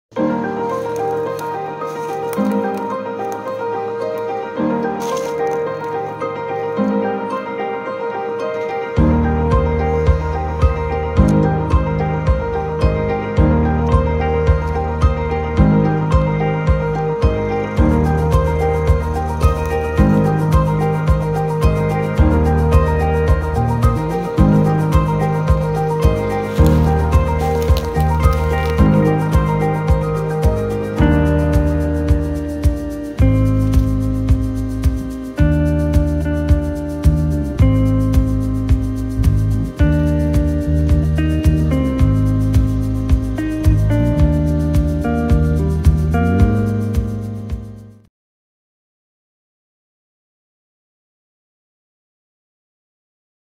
Rc rock crawler homemade backyard crawler course the making.
Ok my 7 year old is bugging me to make this so im looking for help lol there's alot of inspirational videos on here hopefully im half as creative. Cheers from pei.